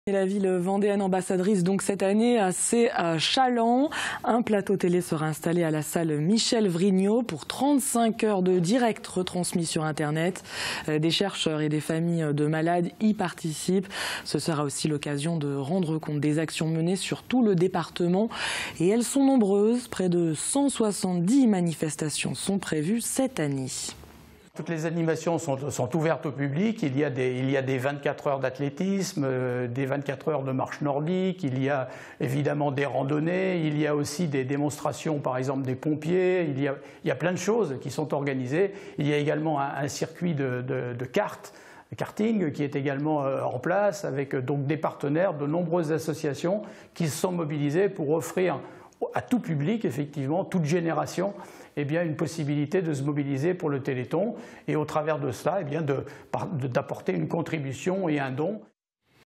Téléthon 2016 : Réaction de Daniel Gaboriau (Vendée)
Daniel Gaboriau, coordinateur du Téléthon en Vendée, fait le point sur cette initiative de l'association "AFM-Téléthon" qui se déroulera à Challans. Pour exploitation TV, droits réservés, nous contacter